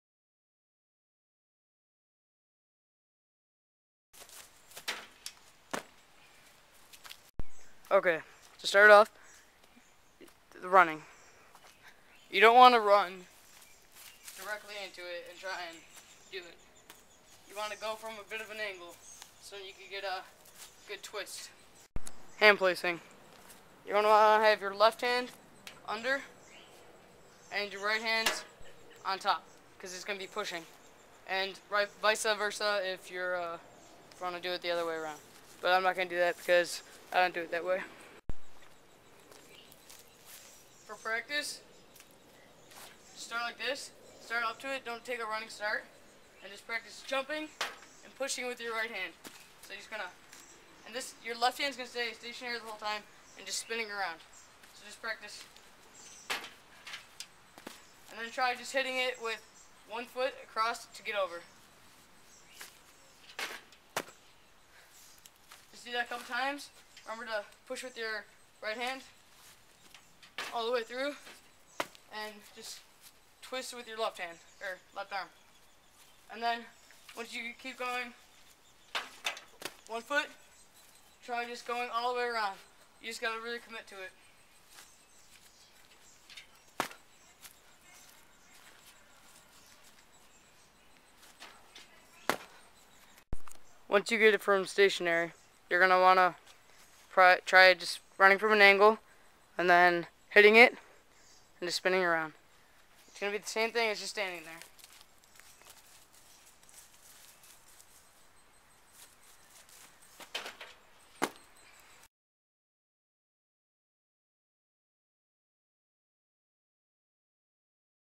Cool Parkour Turn Tutorial (palm spin)
In this How To video i show you guys how to do a cool parkour turn!!
Its easy once you get the hang of it and i go step by step to show you haw to do it